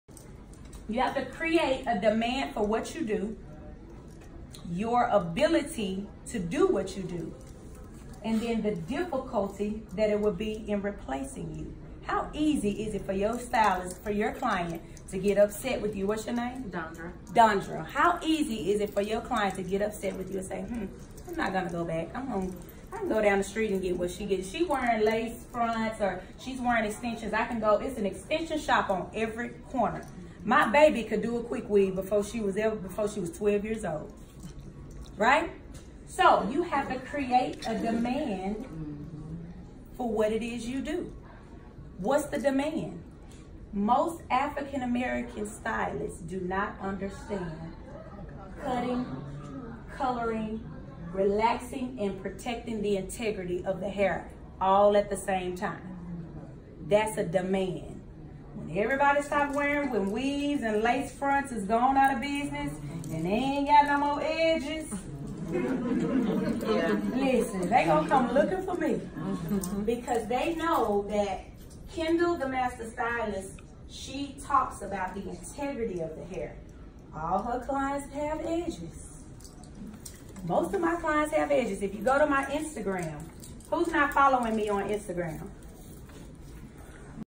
How to become a 6 figure stylist.
There are 3 🔑 points to becoming a booked hairstylist in season and out of season.

🔑CREATE A DEMAND FOR WHAT YOU DO.
🔑YOUR ABILITY FOR WHAT YOU DO.
🔑THE DIFFICULTY IT WOULD BE TO REPLACE YOU.
The Hairstylist Master’s Class was a packed house at the @texashairshow over the weekend!

Book your next coaching course with Kendall and become a stylist in demand!